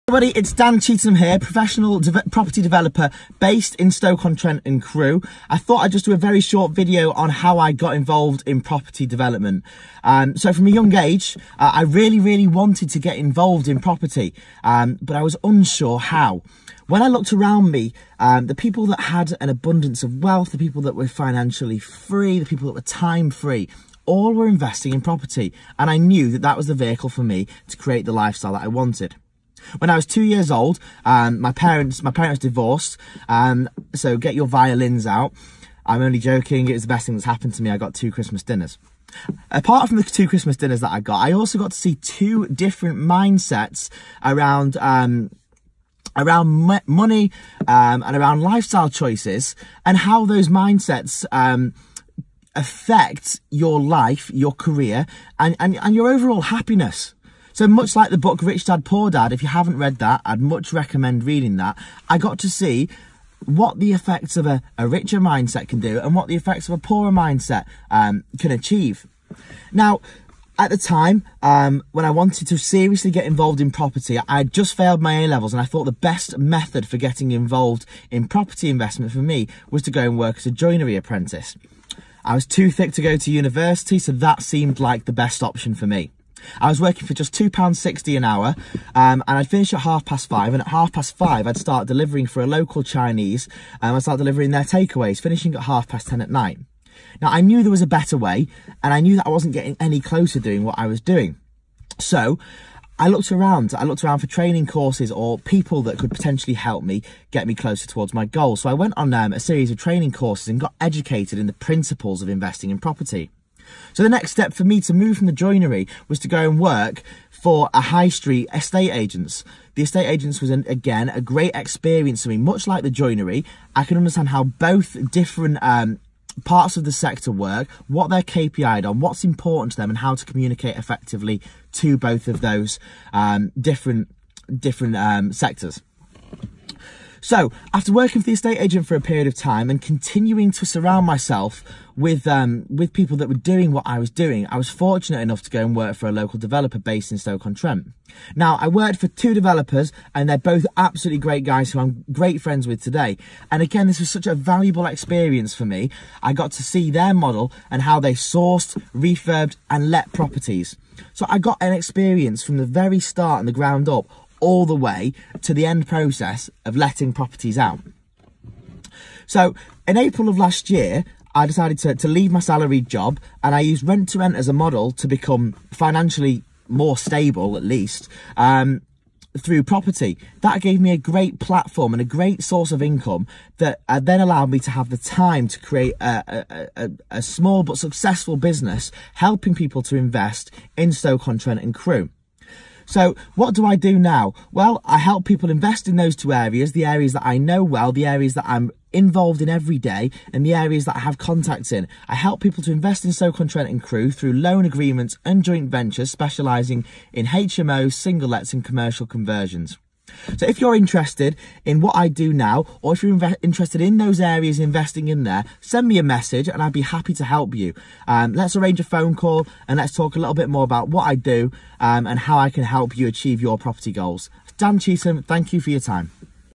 How I started as a property developer
Here’s my short story about how I started as a property developer in stoke on Trent and Crewe